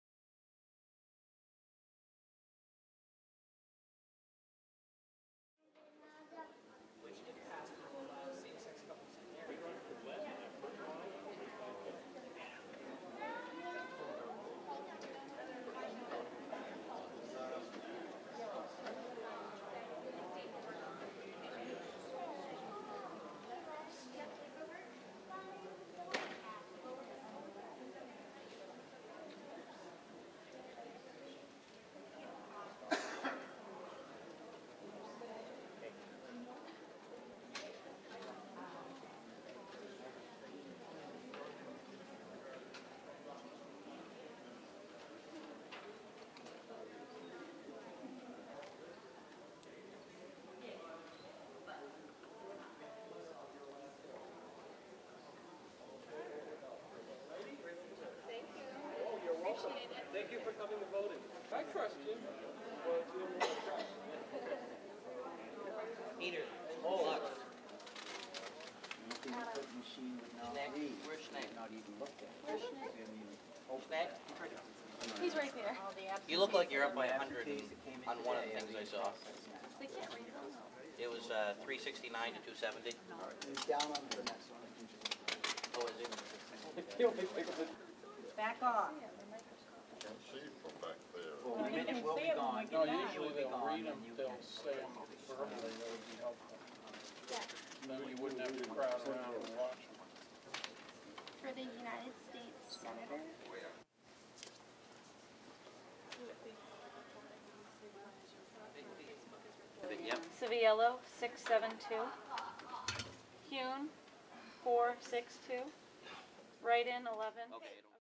Election Day 2012 Bangor Maine
Although the polls have closed, newly registered voters in Bangor, Maine, are allowed to cast their ballots after waiting in line for more than an hour.